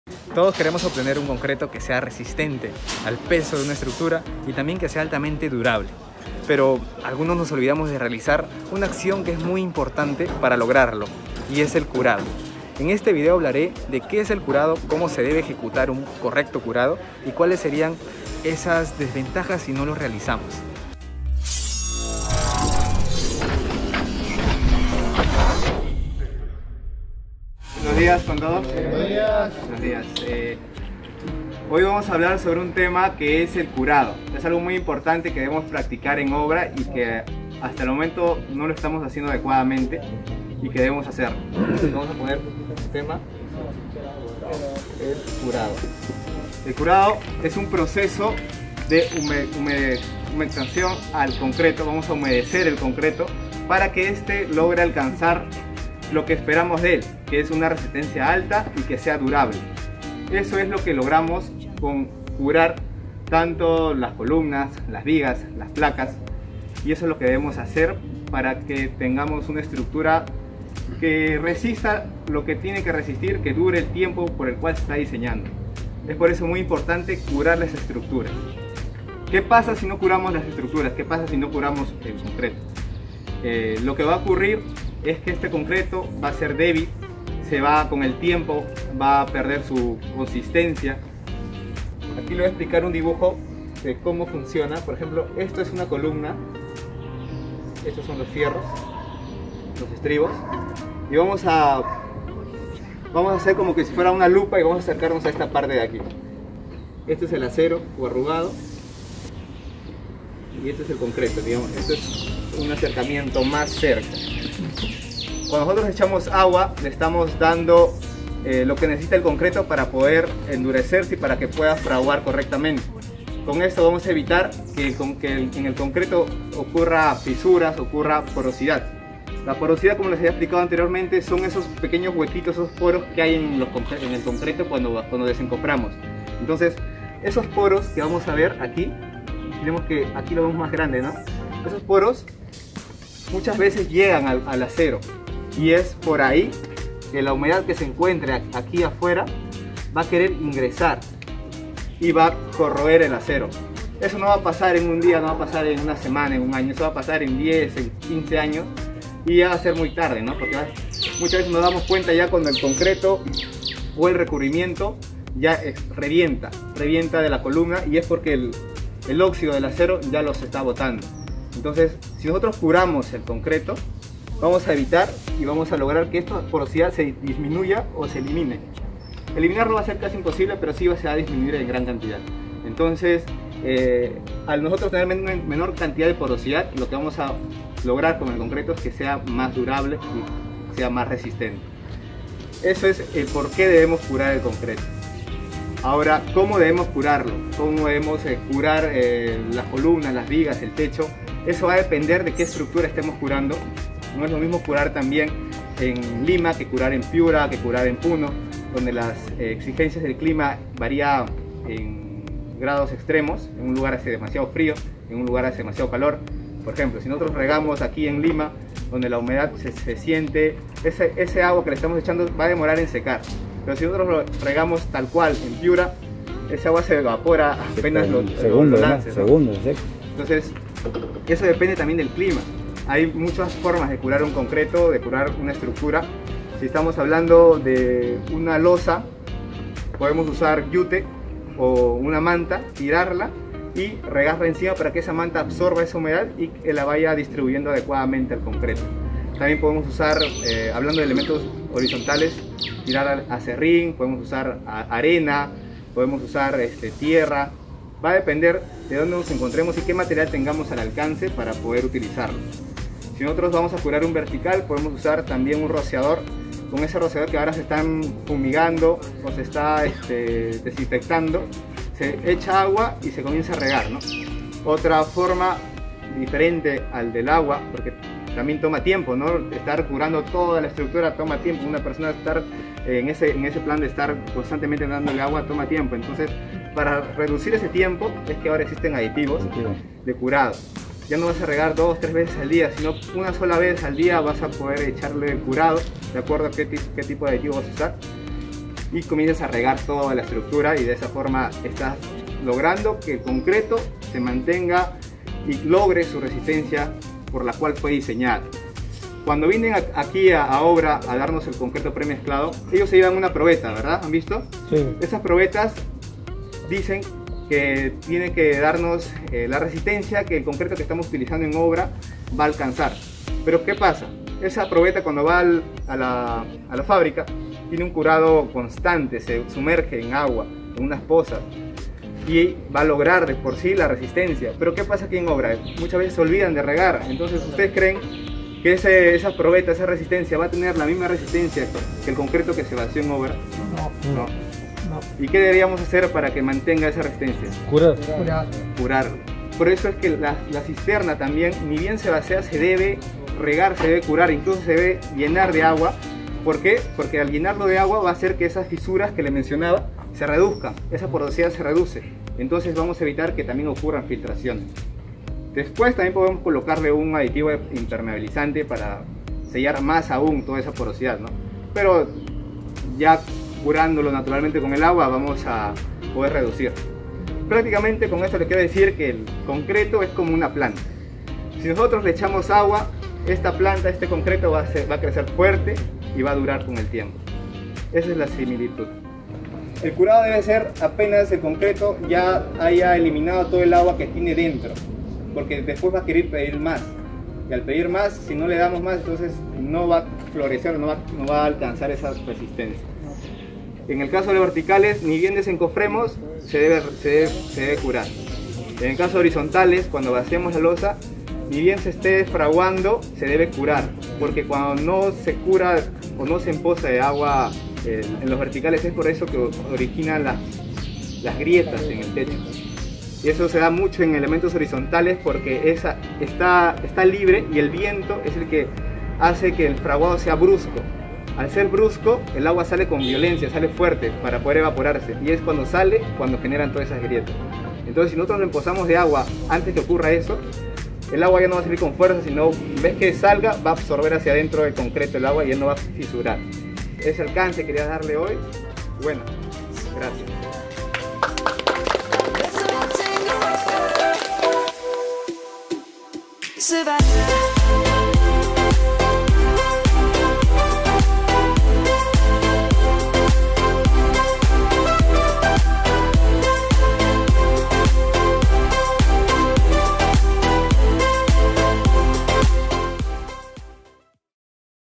CURADO DE CONCRETO | POR QUÉ ES MUY IMPORTANTE HACERLO Y QUE PASARIA SI NO LO REALIZAMOS
En este video hablare de: ¿Qué es el curado?, ¿Cómo debemos realizar un correcto curado?, ¿Cuándo debemos curar? y ¿Qué pasaría si no lo realizamos correctamente?

Comparto con todos ustedes las charlas que tengo todos los días con los constructores, con el único objetivo de que juntos podamos aprender algo nuevo cada día.

Un abrazo.